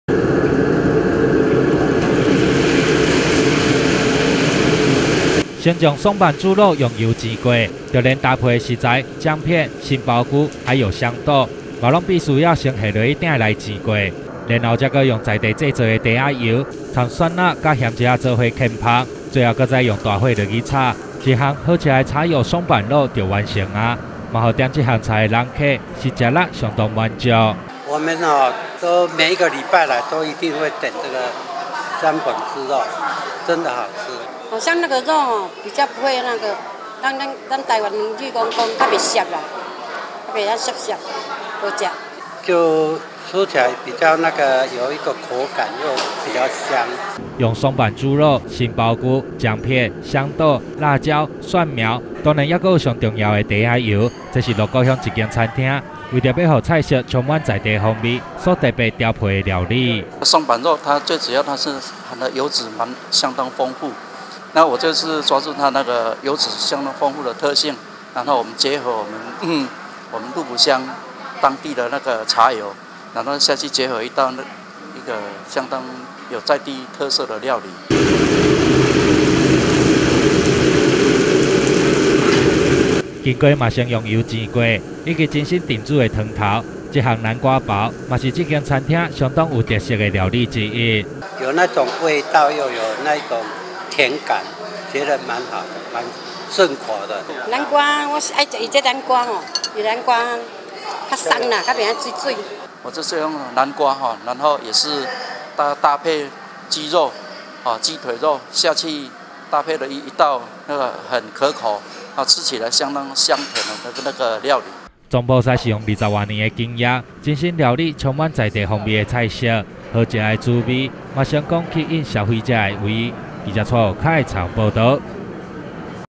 南投新聞-鹿谷茶油松阪肉
鹿谷鄉有一間經營20多年的餐廳，為了讓菜色充滿在地特色，業者是用松阪豬肉、杏鮑菇、甜豆、薑片等食材，再加入當地製作的茶油下去大火快炒，炒出來的松阪肉是香Q又有嚼勁，另外，還有廚師運用獨家配方料理的南瓜褒，口味也是一流，是讓許多遊客都紛紛上門，要來吃這充滿在地風味的美食料理。